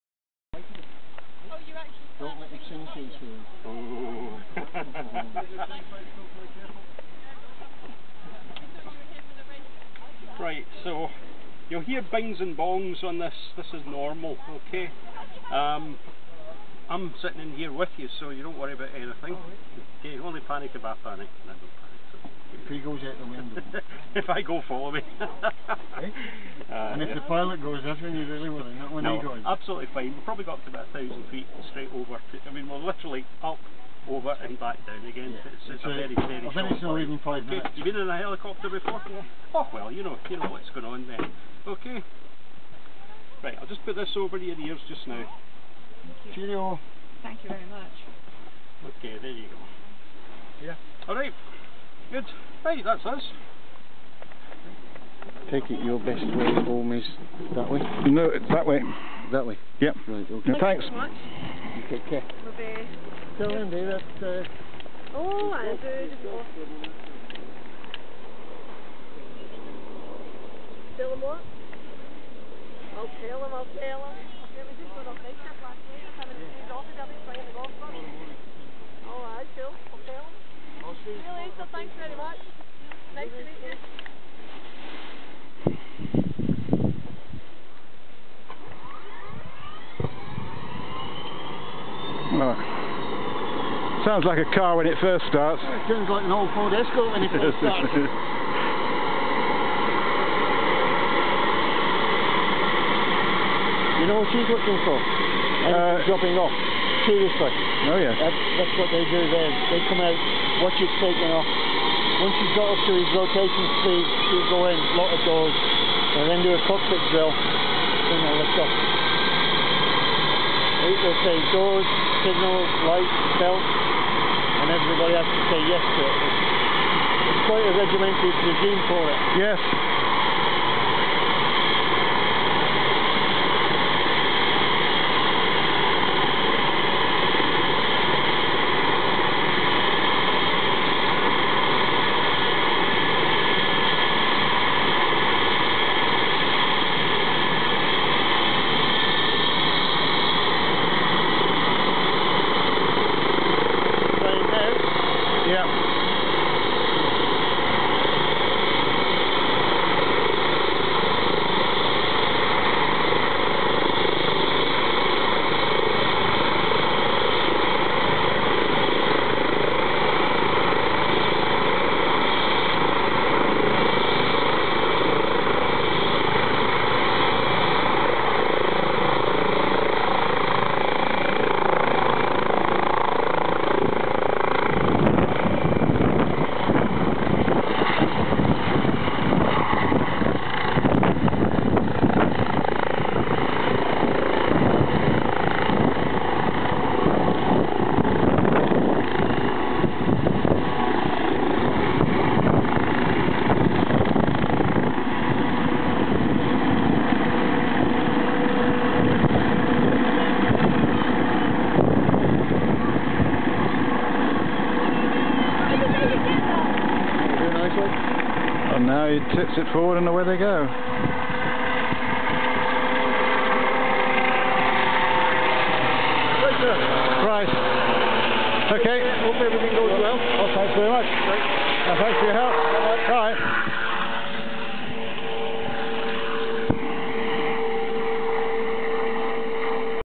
P1030078cut.MOV
Celia is flown off to the Royal Infirmary